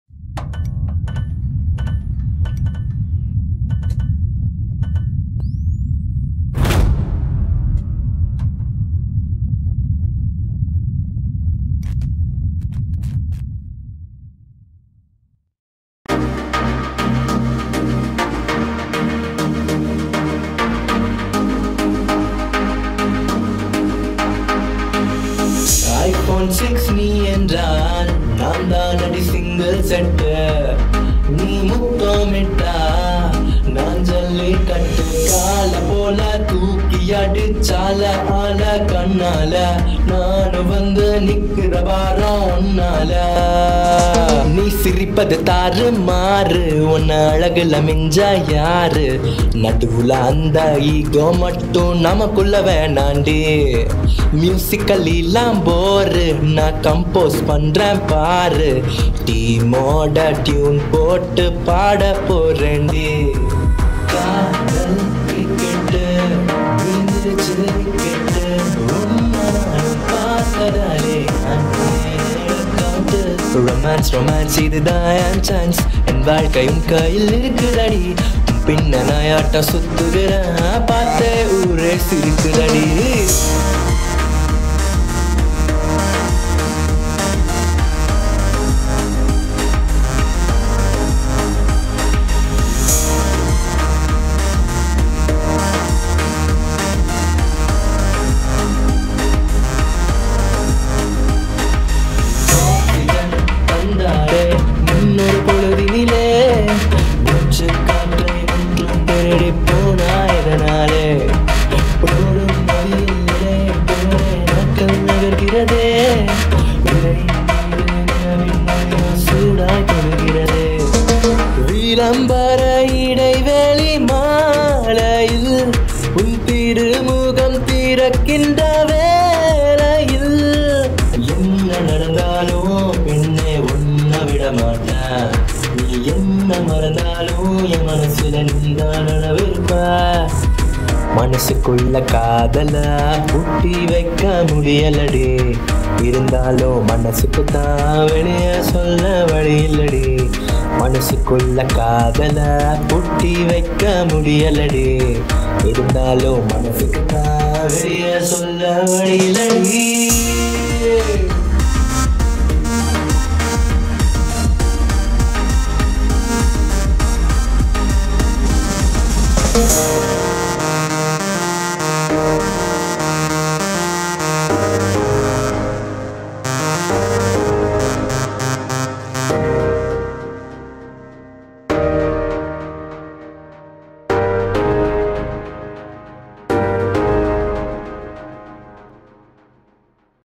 Hip Hop Tamizha Mashup | Fly Studios | Surya darshan | Neruppu Kishore
Finally, Its there. 😊

HIP HIP TAMIZHA MASHUP

An awesome mashup by our talents.

Vocals : Neruppu Kishore

Cuts     : Surya darshan

Hope u all enjoy about this. Showing ur love by sharing our upload in social media. Thats help us to grow.😊